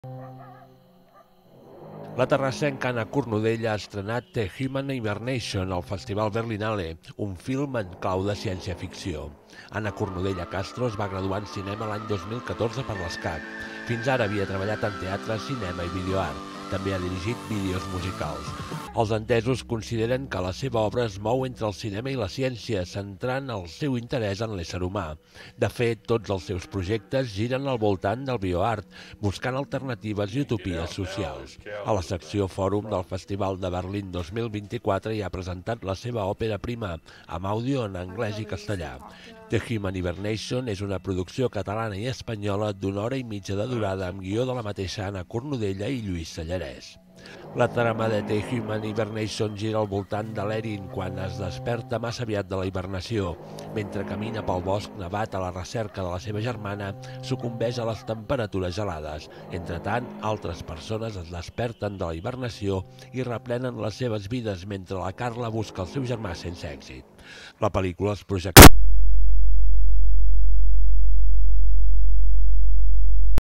Anna Cornudella estrena 'The human hibernation' al Festival Berlinale
La pel·lícula es projectarà a Berlín fins el dia 25 de febrer